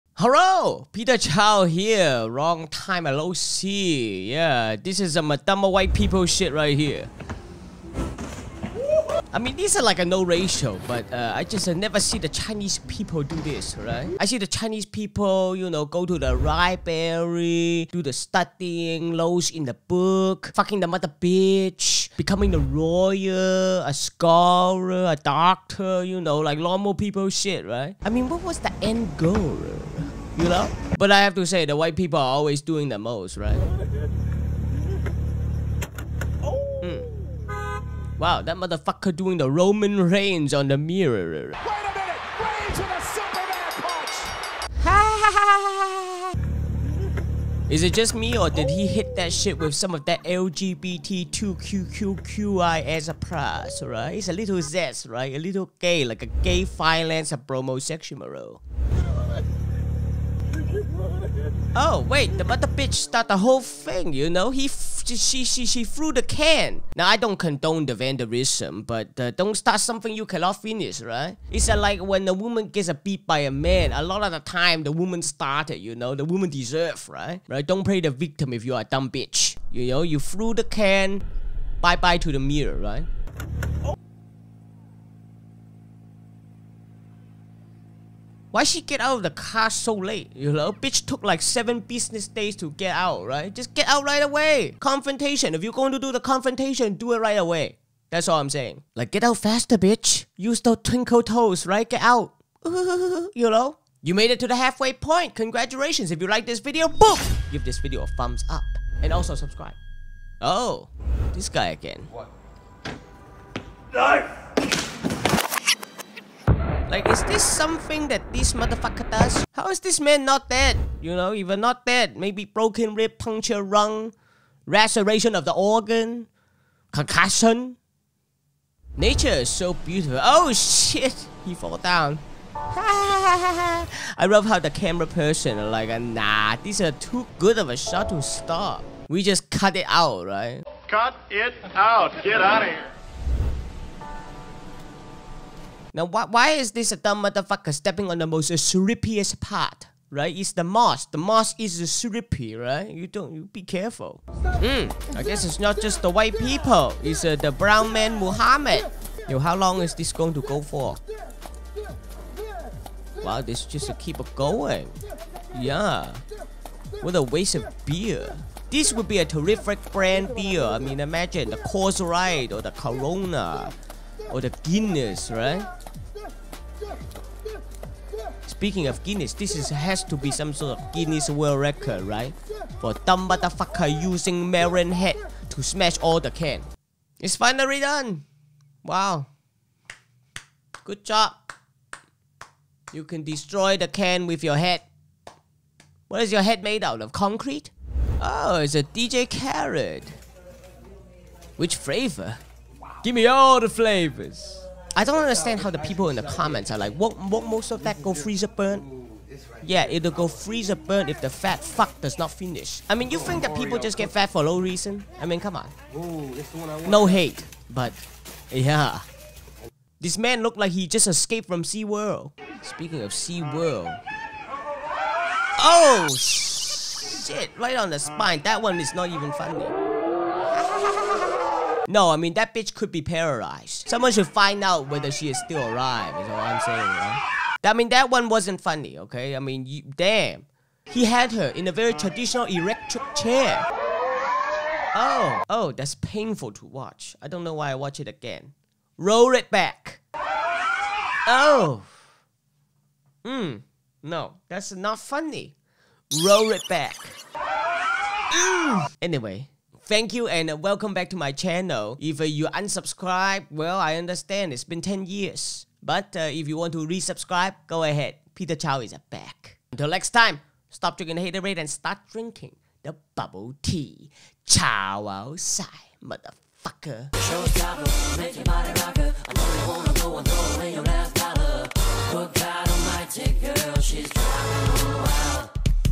Peter Chao Reacts to White People Doing Things
Peter Chao returns to YouTube after ten years to celebrate the evolution of White people.